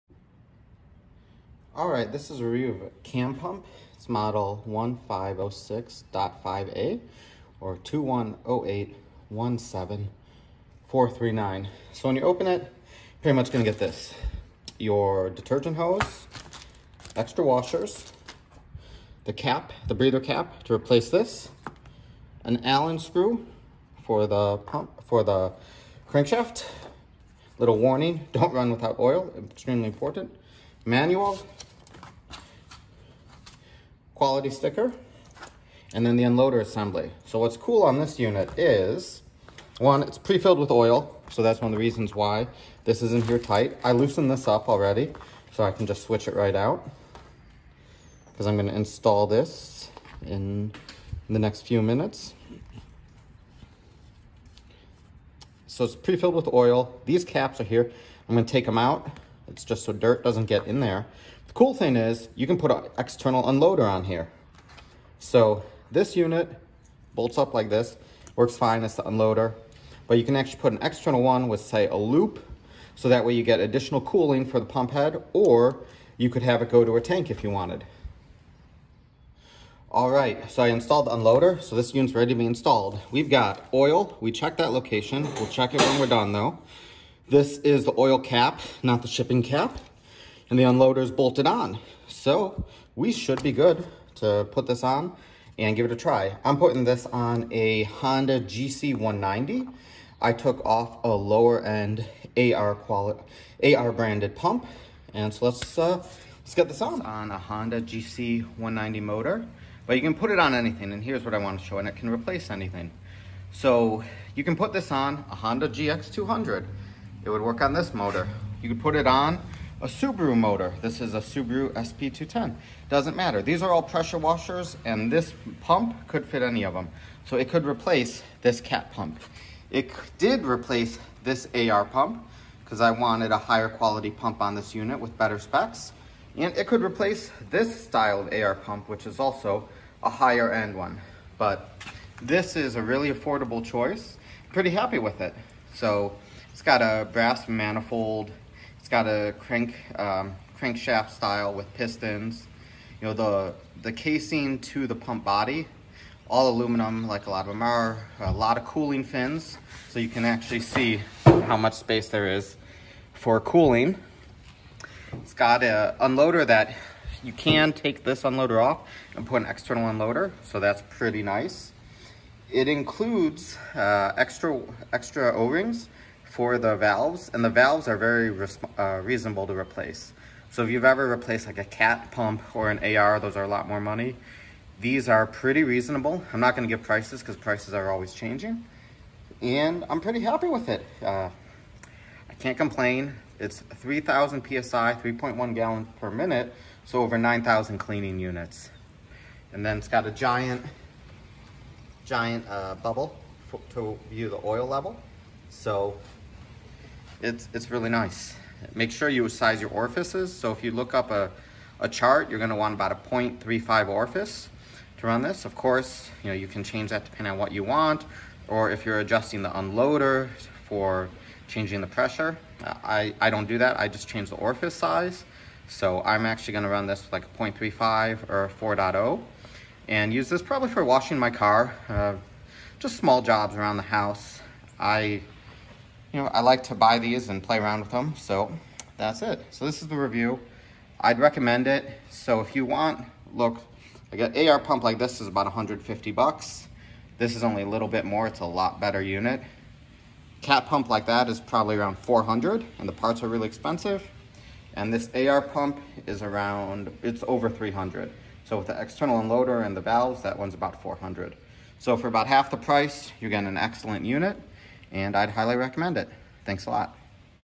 CanPump 3000PSI – 3.1GPM Triplex Pump 3/4″ Hollow Shaft 3WZ-1506.5A Review and thoughts
I received this pump a few weeks ago from my friends at Canpump.  Here is a quick overview and a look at what other pumps it can replace.

CanPump Amazon Store: